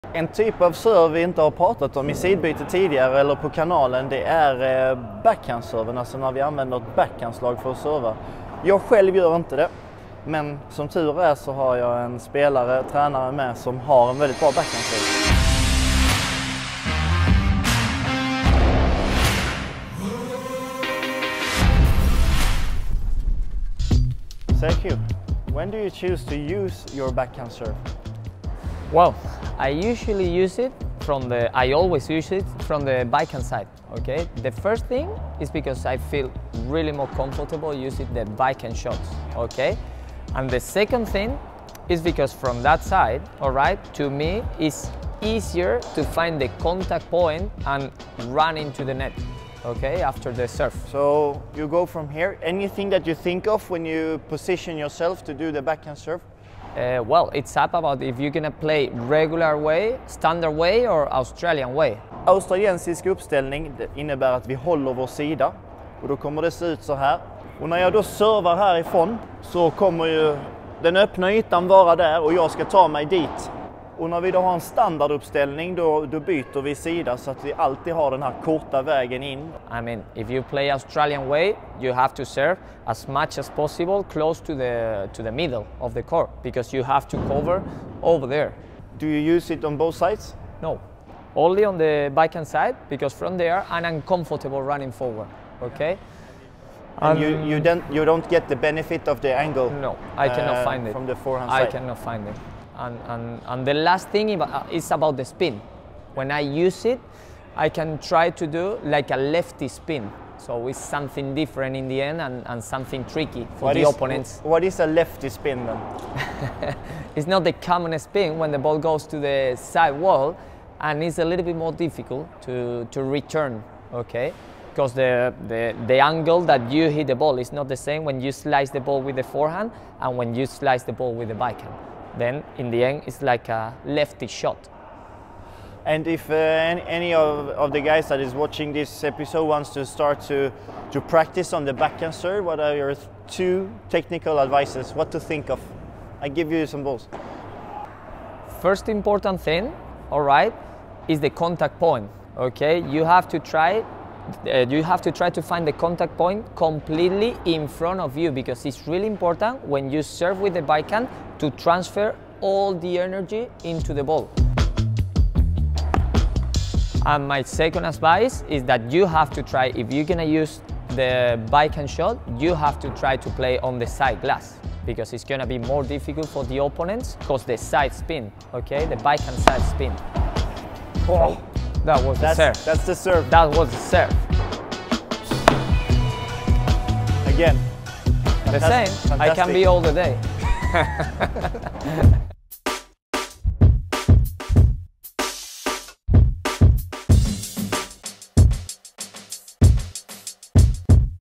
TEKNIK - Tipsen för backhandserven
I sidbyte med Andréas Johansson går vi igenom tips och råd för teknik, taktik, positioner och ja, allt som kan hjälpa dig i att utveckla din padel.

I detta avsnitt guidar Sergio Beracierto er igenom backhandserven. Varför använder han den och vilka tips har han?